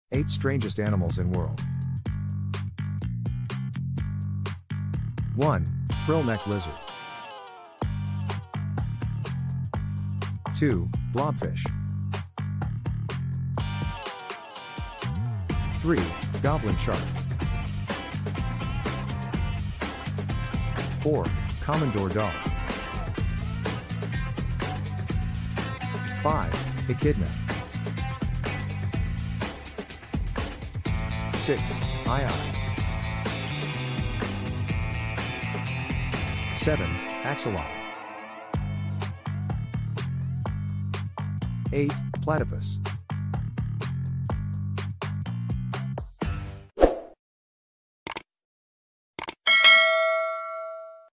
The 8 Strangest Animals You Didn't Know Existed
Do you think you've seen it all when it comes to animals? Think again! This video showcases some of the strangest creatures on Earth, from the spiky-tailed iguana to the proboscis monkey .You won't believe your eyes!From giant isopods to tardigrades, these are some of the most bizarre animals in the world. This video will introduce you to ten of the strangest creatures on our planet, and might even make you appreciate the more mundane animals in your life!